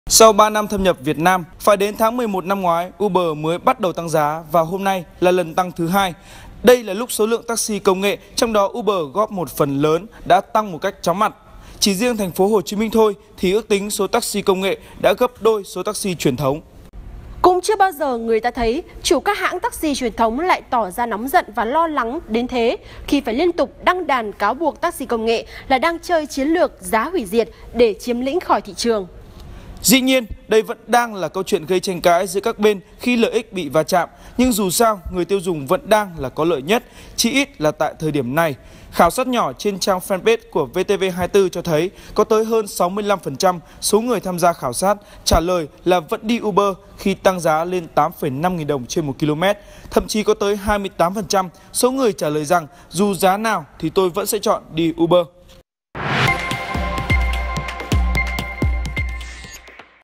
Uber tăng giá, bạn vẫn tiếp tục sử dụng dịch vụ này chứ? - Tin Tức VTV24
Mặc dù Uber thì đã tăng giá nhưng có thể dễ dàng thấy rằng có rất nhiều người vẫn muốn sử dụng dịch vụ này, thậm chí có tăng giá nữa cũng không sao.